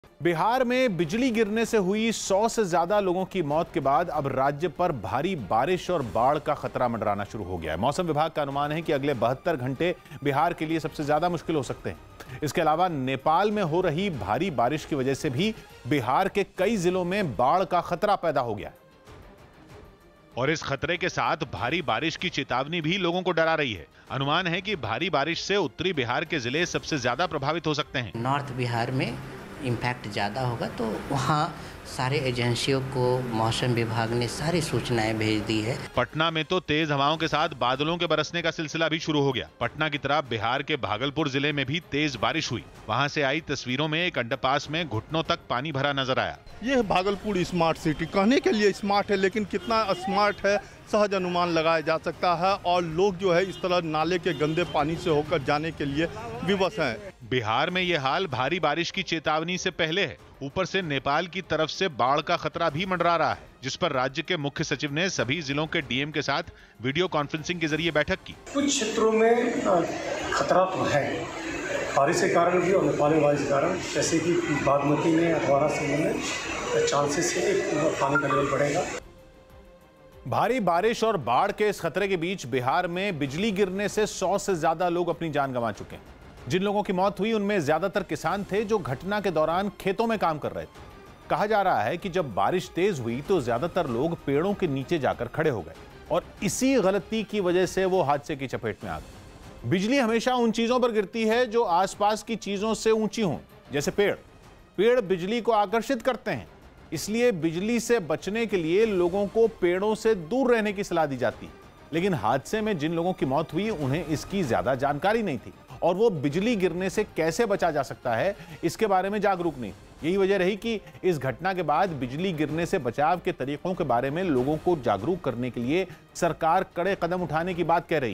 Bihar पर मंडराया बड़ा खतरा,भारी बारिश और बाढ़ की चेतावनी | News18 India
पिछले चार दिनों से नेपाल  (Nepal) के कई इलाकों में लगातार बारिश से बिहार की कई नदियों के जलस्तर में वृद्धि होने लगी है. हालांकि प्रदेश की सभी नदियां फिलहाल लाल निशान से नीचे बह रही हैं, पर सूबे में बाढ़ का खतरा (Fear of Flood) मंडराने लगा. मौसम विभाग (Weather department)की ओर से 29 जून तक भारी बारिश के अलर्ट से मुजफ्फरपुर जिले के कटरा, औराई और गायघाट के बाढ़ प्रभावित इलाके के लोग चिंता में हैं.